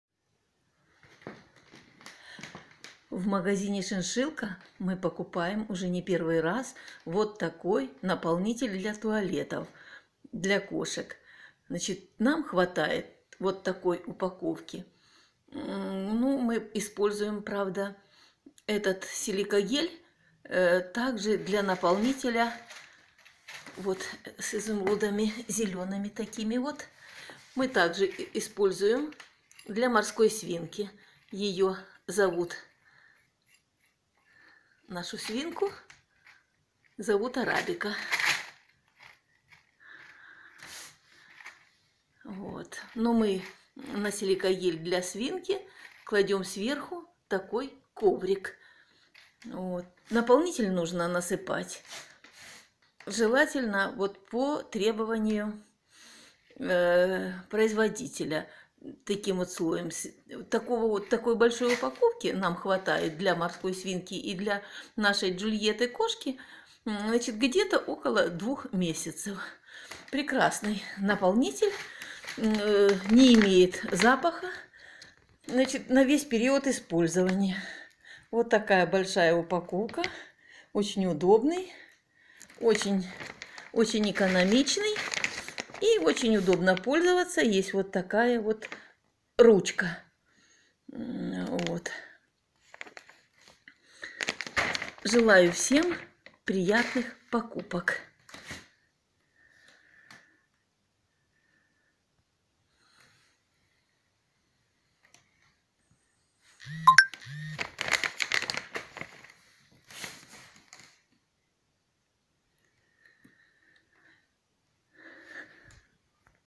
Силикагель 10,5 л AnimАll "Кристаллы изумруда"
Наполнитель для кошачьих туалетов на основе силикагеля TM AnimAll немецкой компании Animall International GmbH. Силикагель ТМ AnimAll – это высушенный гель поликремниевой кислоты (SiO2), который химически состоит из того же соединения, что и обычный песок, но является высокопористым телом, поэтому имеет огромную площадь поверхности (800 м²/1 г).